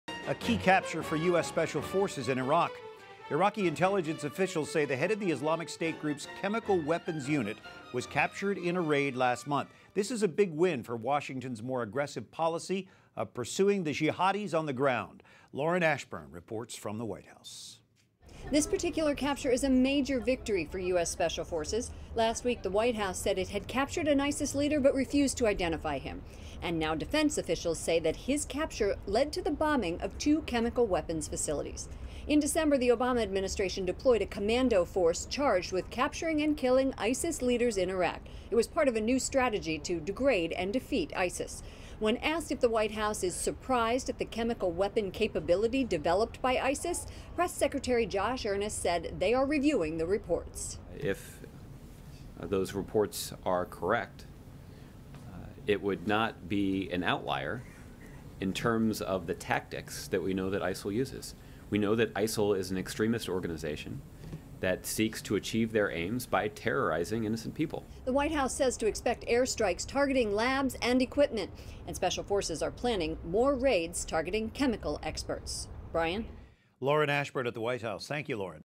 U.S. Captures ISIS Leader
U.S. Special Forces in Iraq capture an ISIS leader -  Lauren Ashburn reports from The White House.